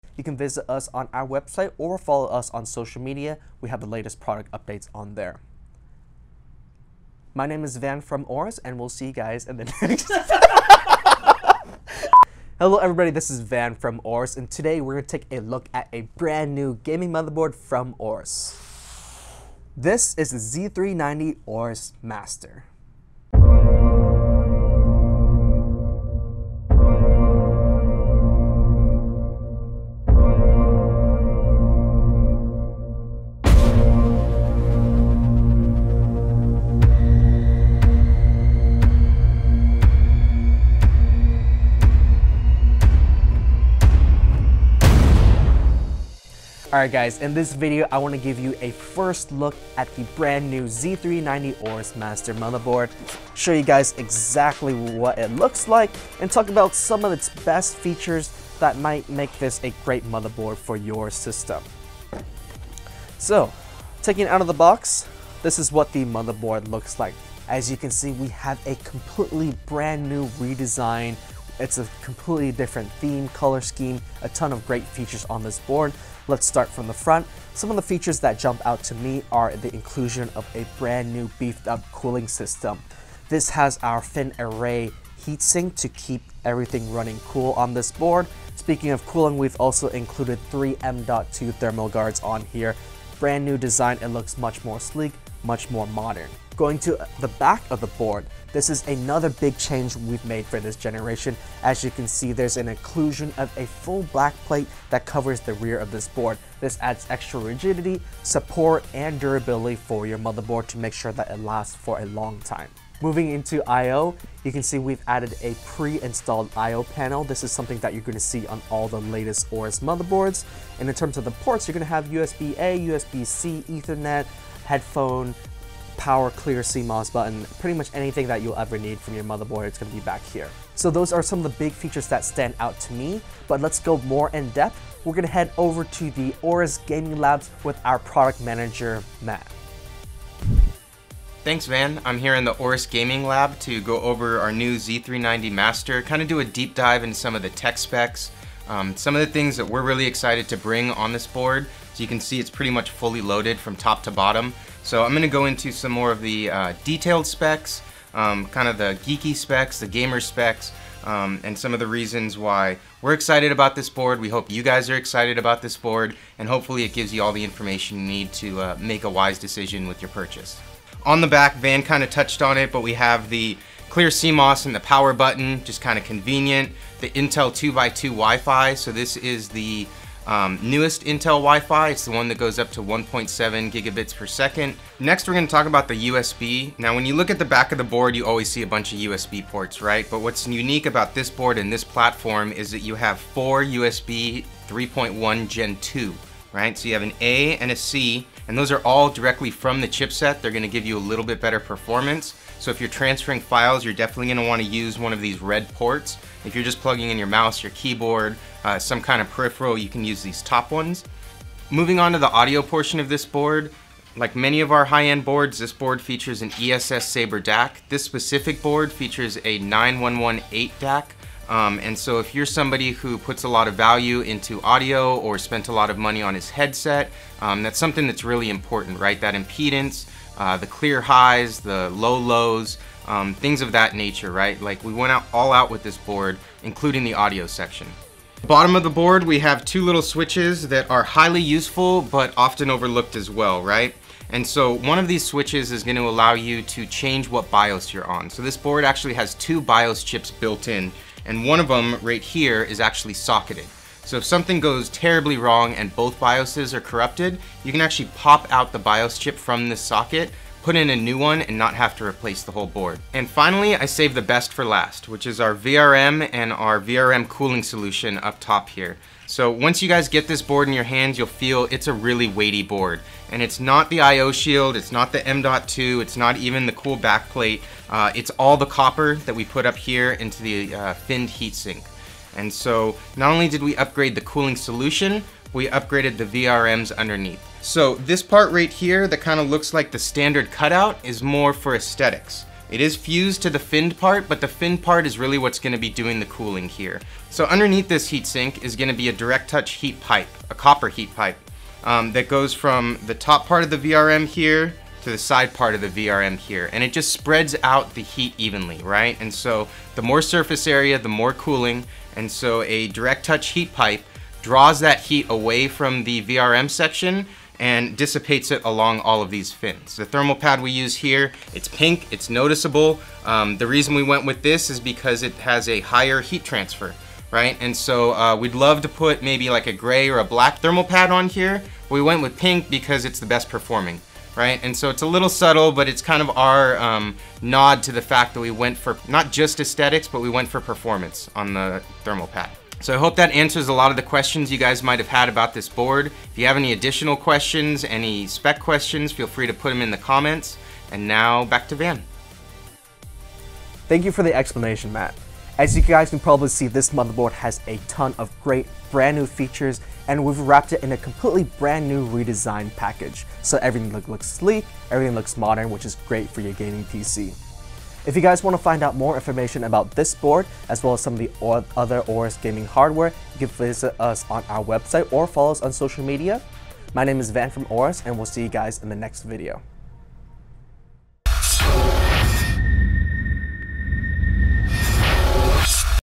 Z390 AORUS MASTER | Product Overview
The next generation is here! The new AORUS Z390 Motherboards have launched today! Watch as Van & Matt explain some cool and new features about the AORUS Z390 Master! Now available!